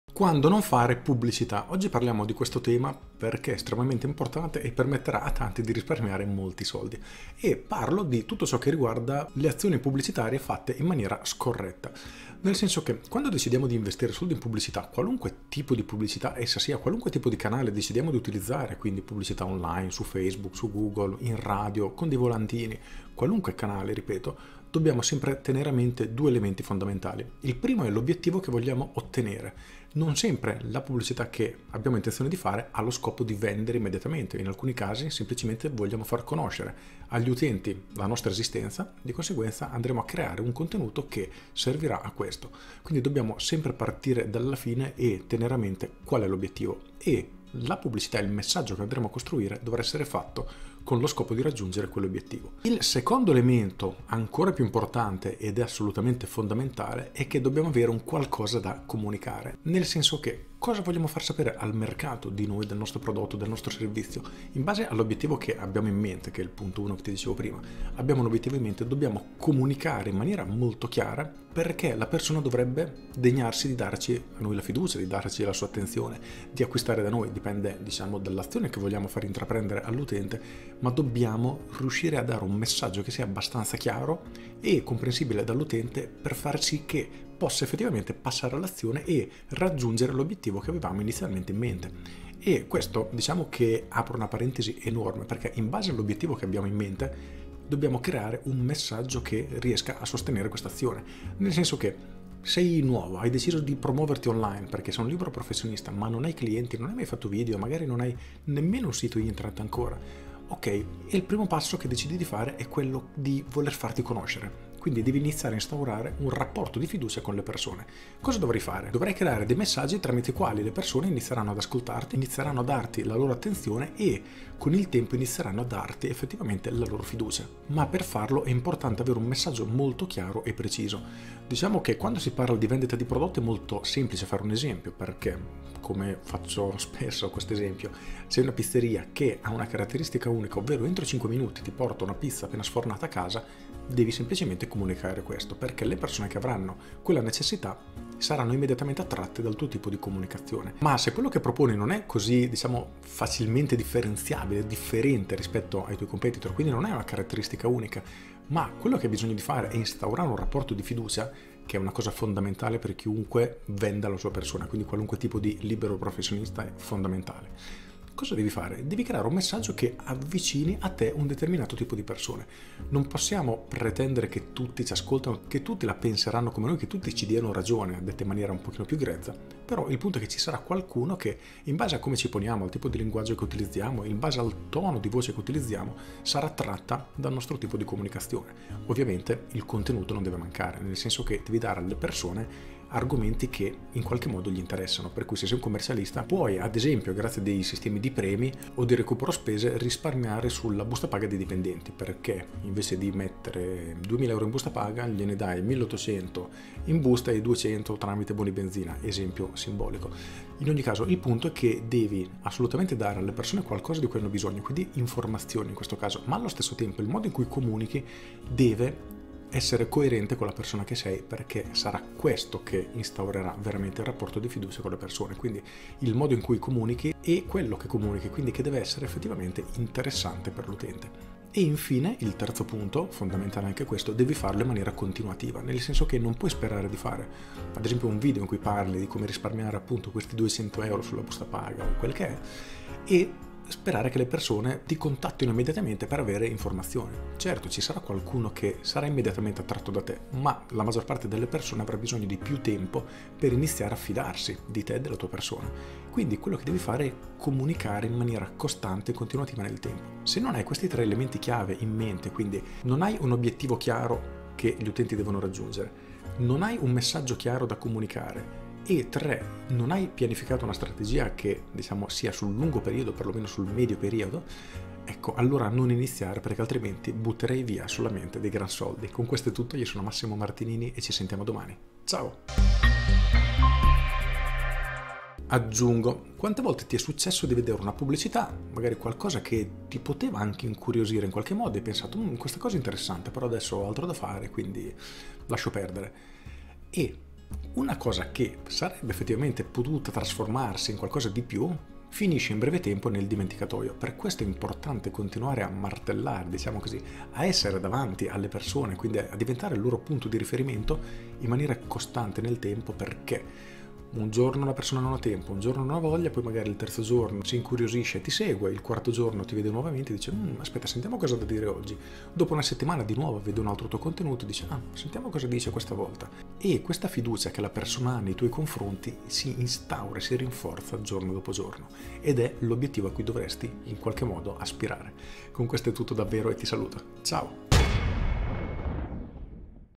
Quando non fare PUBBLICITÀ
Vuoi scoprire di più? Clicca qui

CAPITOLI DEL VIDEO: quando non fare pubblicità
0:00 INTRODUZIONE
0:24 I 2 elementi fondamentali
0:55 Come comunicare nella pubblicità
3:50 Comunicare in maniera continuativa
5:00 Martellare nella comunicazione

Marketing Strategico: il libro